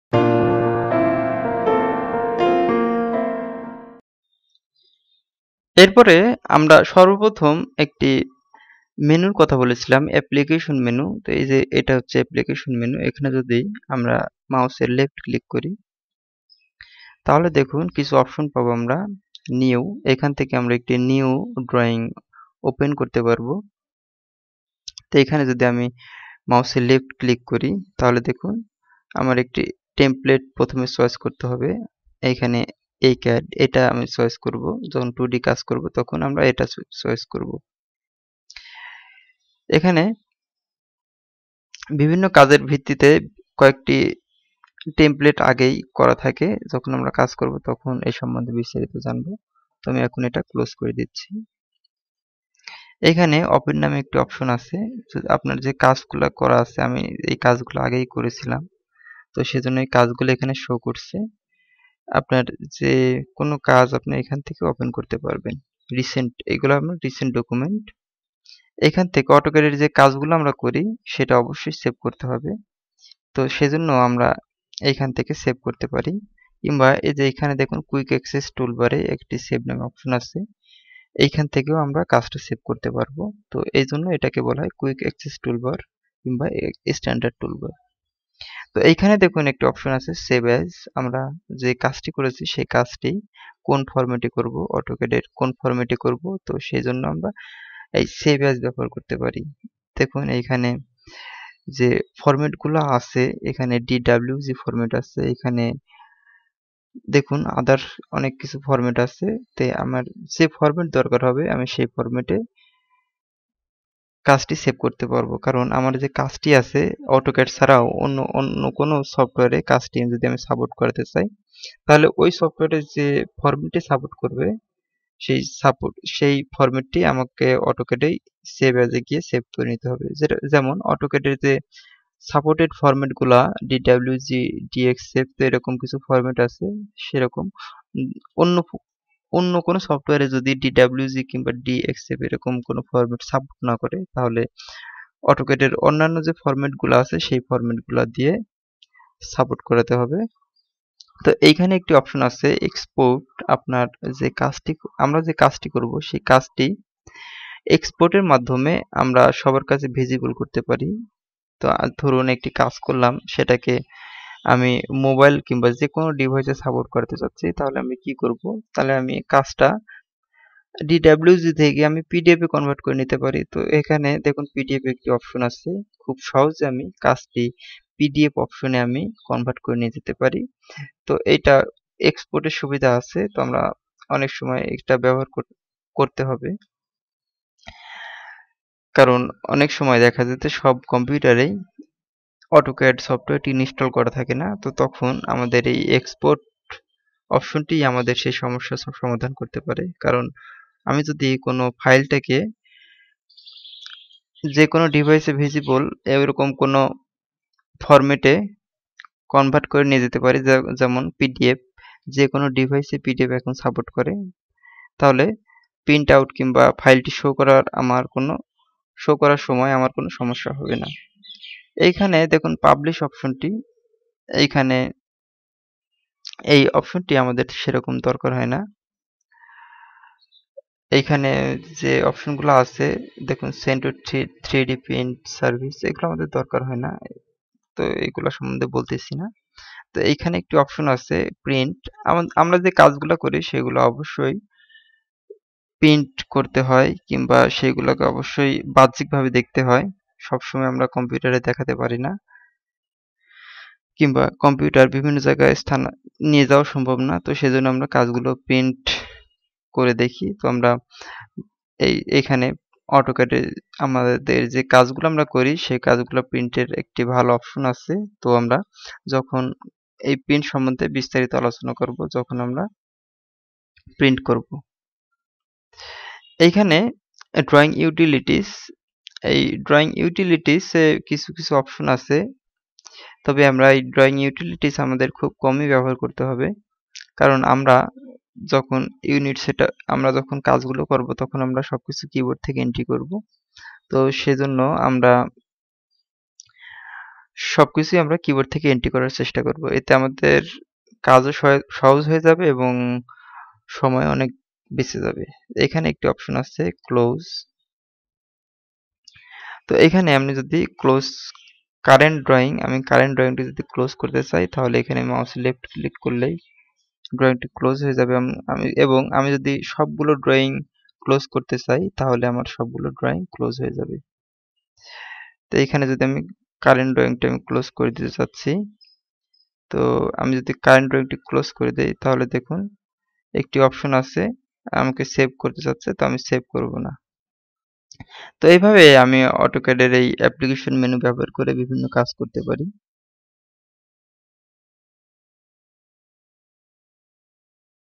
AUTOCAD 2D Bangla Tutorial Part 4 | AUTOCAD Tutorial For Beginners | অটোক্যাড বাংলা
This is autocad basic bangla tutorial for beginner level students.This autocad bangla tutorial about Autocad interface details. Stay with bd cad and learn more about autocad.
অটোক্যাড বাংলা টিউটোরিয়াল পর্ব ৪ এ আপনাকে স্বাগতম